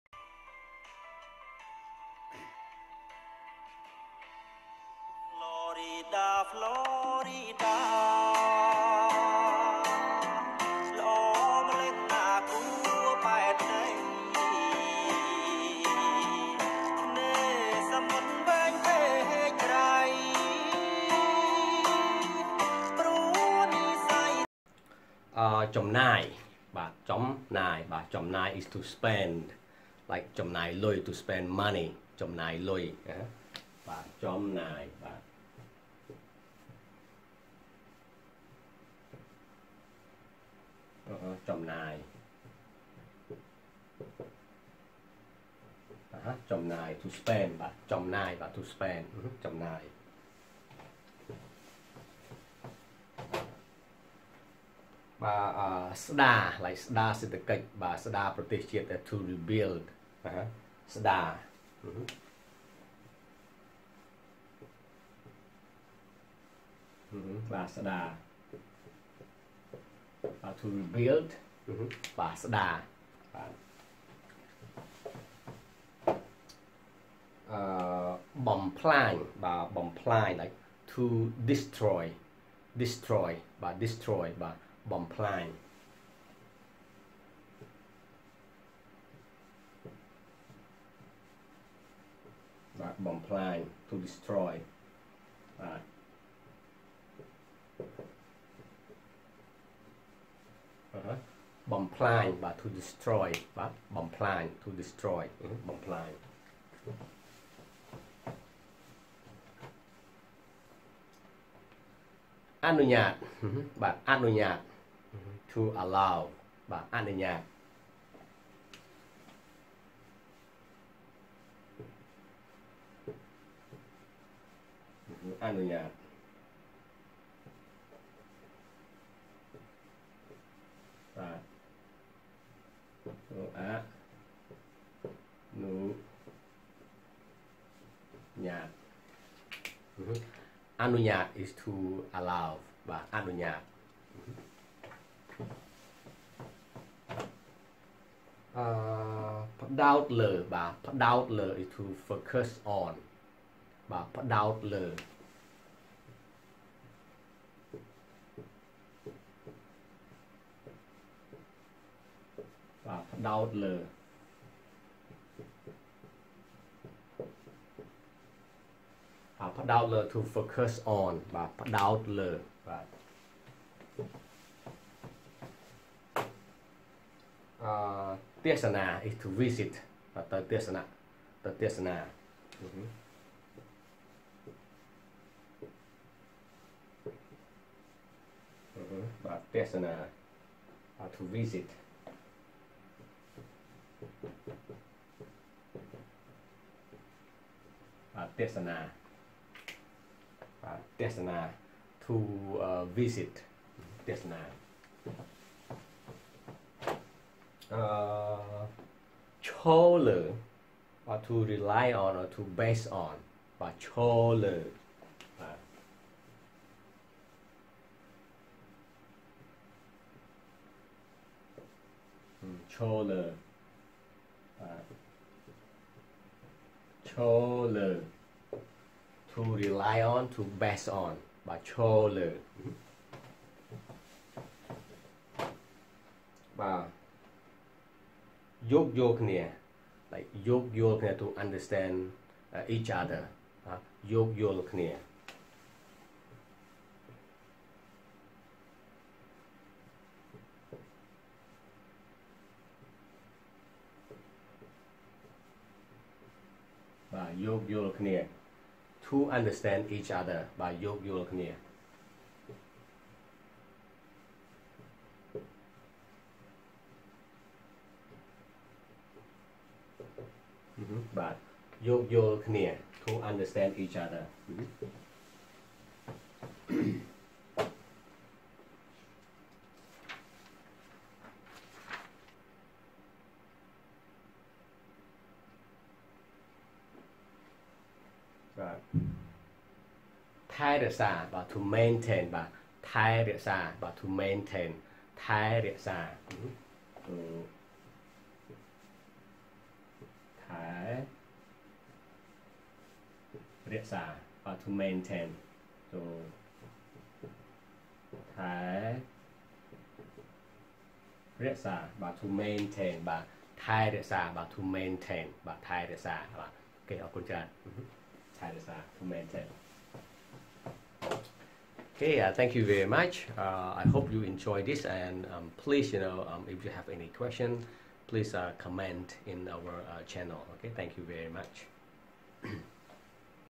Khmer Lesson 11: 200 words related Development
Learning Khmer language ASEAN  languages 200 hundred words related to development, Business, Administration and Development.